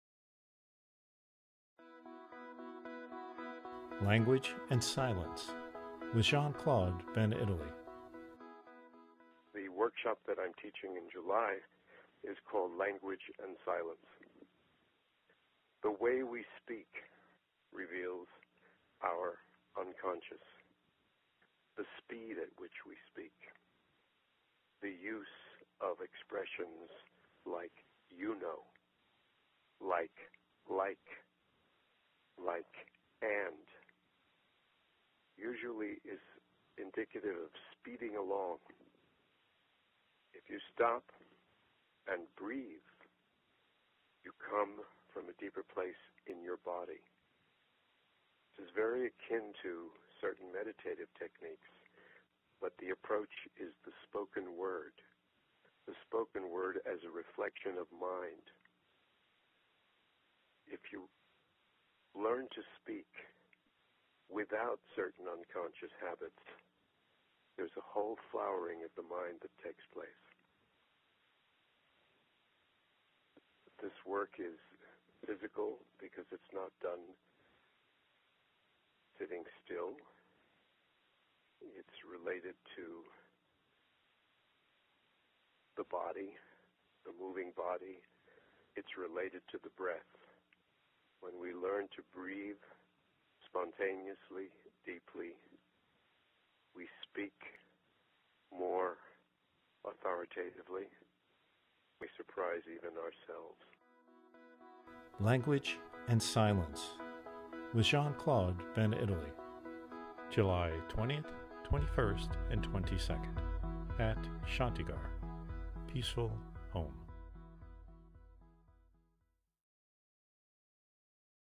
Language and Silence with Jean Claude van Itallie
Jean-Claude van Itallie's workshop Language & Silence offers both traditional and unique techniques to slow down as you think and speak, to plumb your inner depths, to deliberately honor community, and thus to act more spontaneously and effectively in the world.
July 20-22 at Shantigar, a spectacularly beautiful old farm on a mountainside in Western Massachusetts.